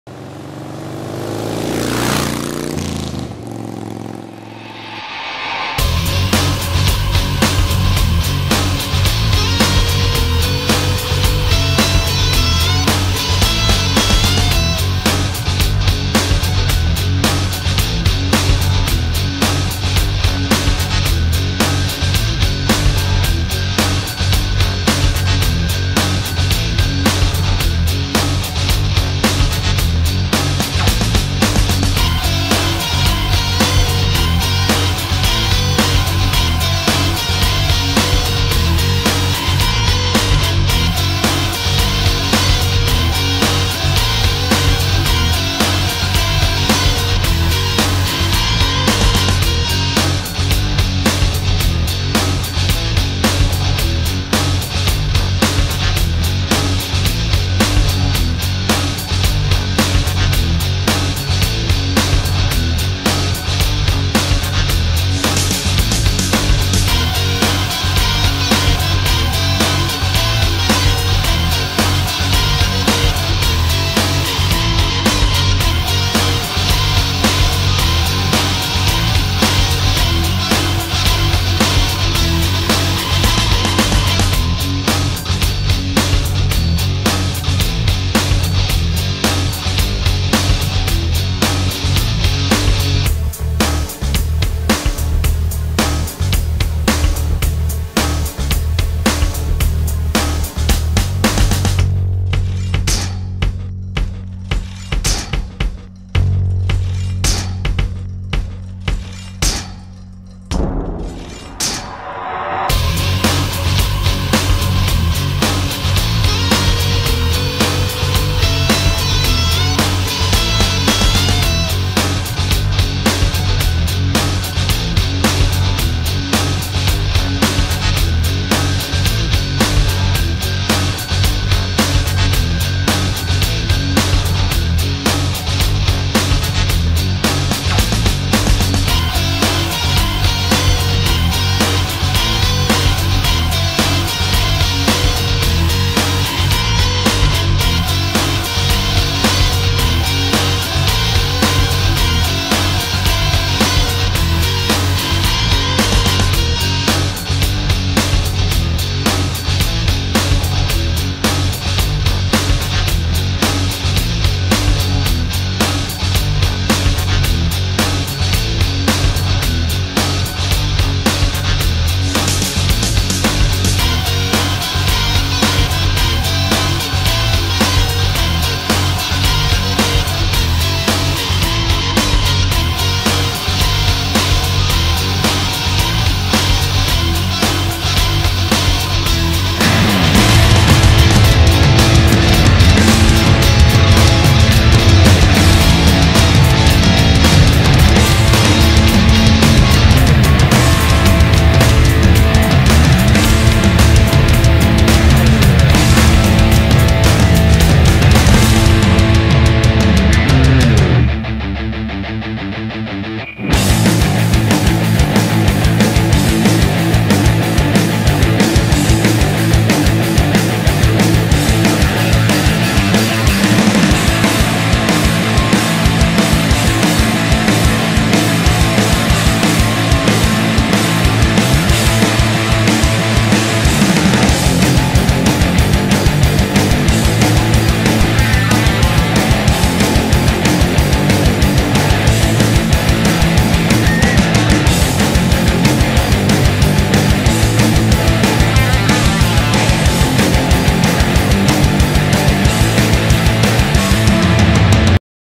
Una domenica al motoraduno di Bertiolo (UD)
Domenica 25 marzo 2018 al motoraduno di Bertiolo (UD) "Cantine & Motori".